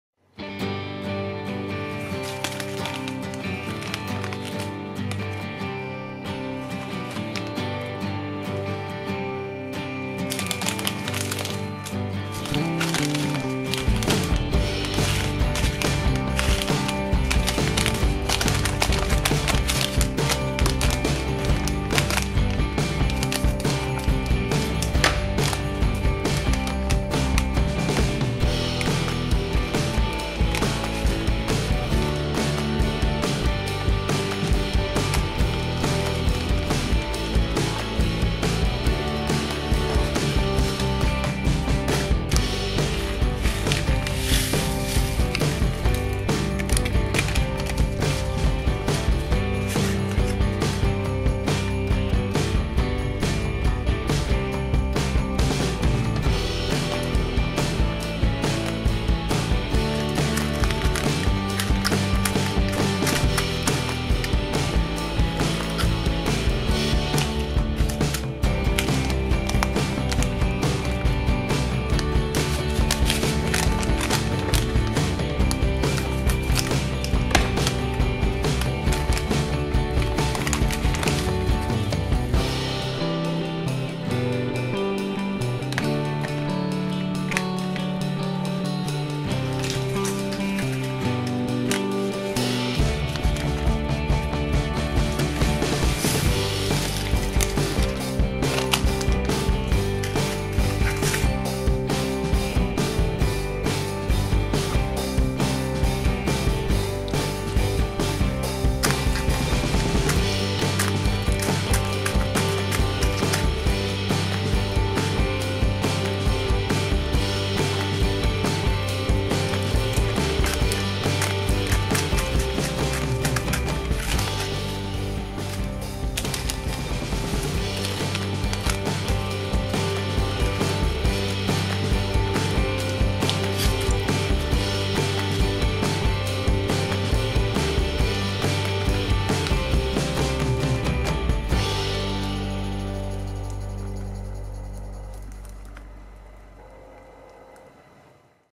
Chupa Chups CRAZY DIPS - Lemon & Cola Flavour
We show Chupa Chups - CRAZY DIPS Foot Lollipop - Lemon & Cola Flavour YouTube Unboxing Video for babies, kids, families & collectors